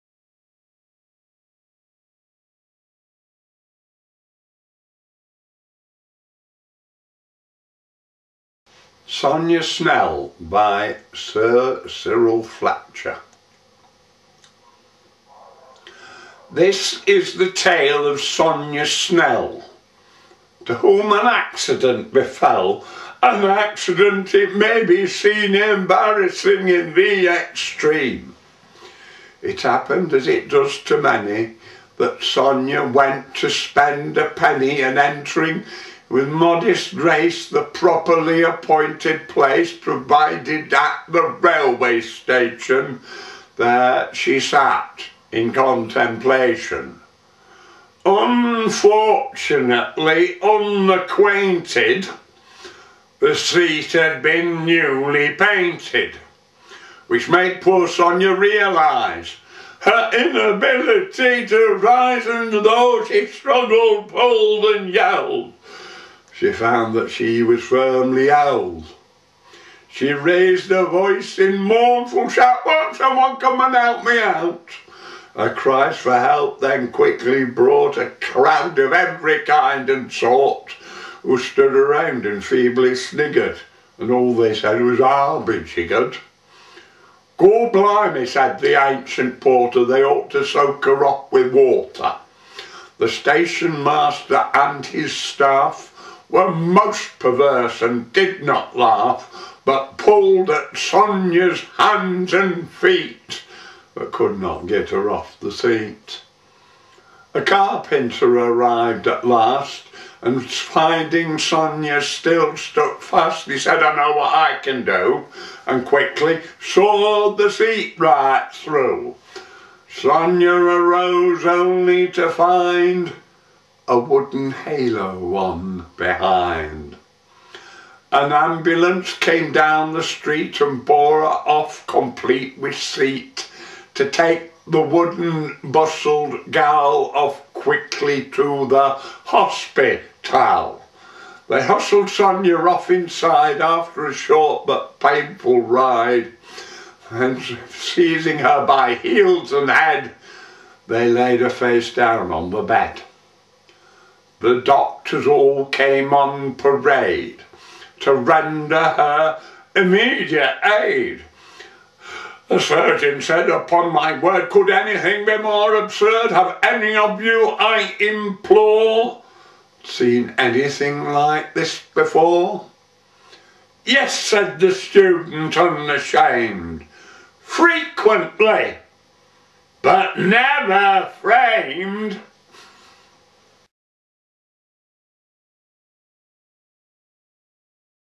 Monologue John Bartley - Sonia Snell
Monologue John Bartley performs 'Sonia Snell' by Sir Cyril Fletcher